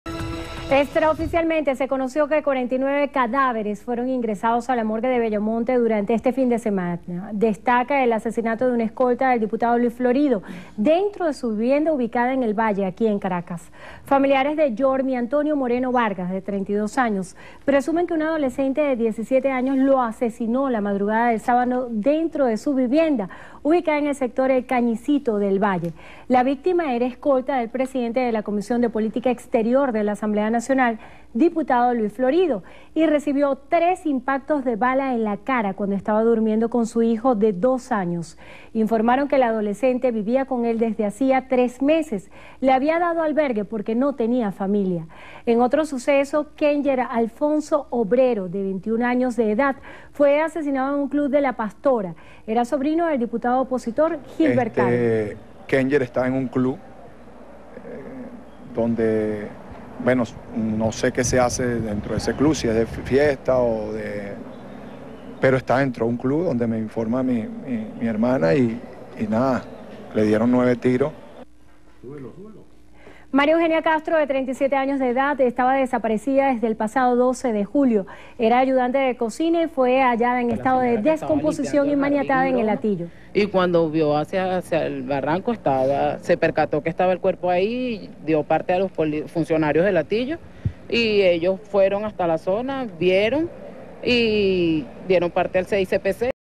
Extraoficialmente 49 cadáveres ingresaron a la morgue de Bello Monte este fin de semana
Extraoficialmente se conoció que 49 cadáveres fueron ingresados a la morgue de Bello Monte durante este fin de semana. Destaca el asesinato de un escolta del diputado a la Asamblea Nacional Luis Florido, dentro de su vivienda ubicada en El Valle, Caracas.